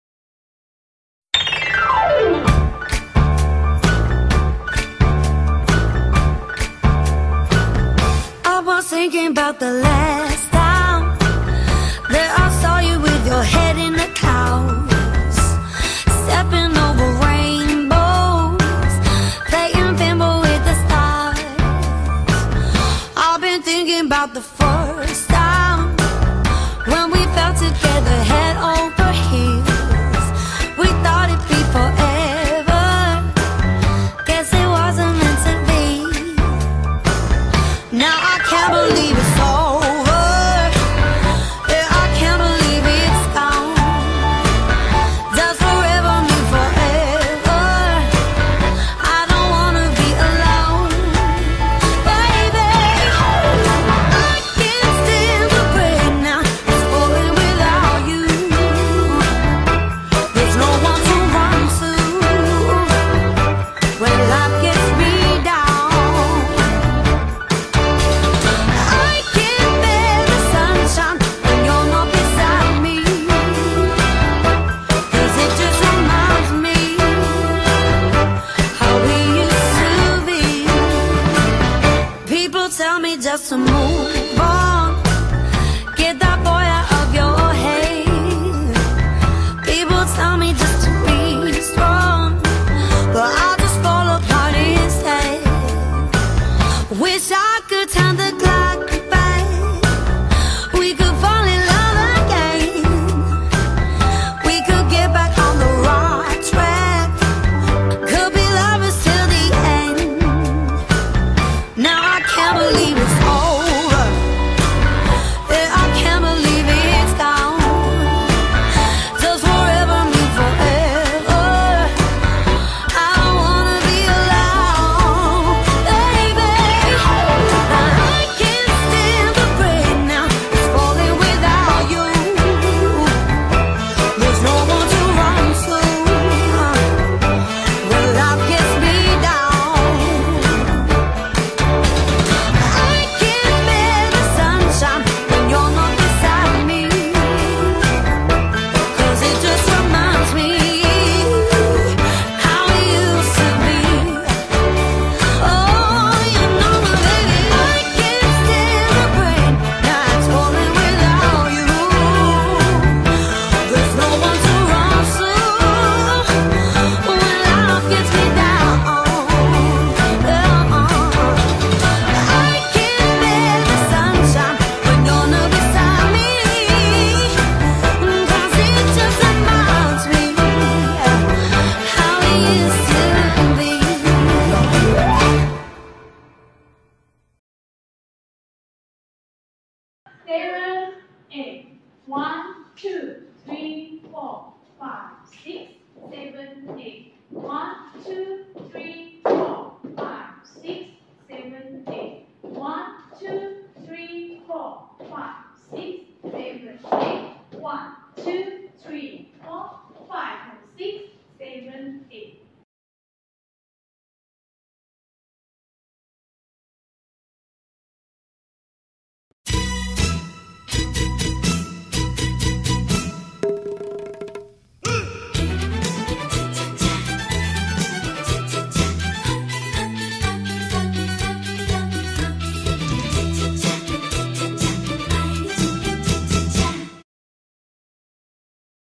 Stepping Over Rainbows AB - Line Dance
BONUS : Breakie 3:33
Choreographer :
Suzi Beau (UK) April 2018
32 count, 4 wall
Absolute Beginner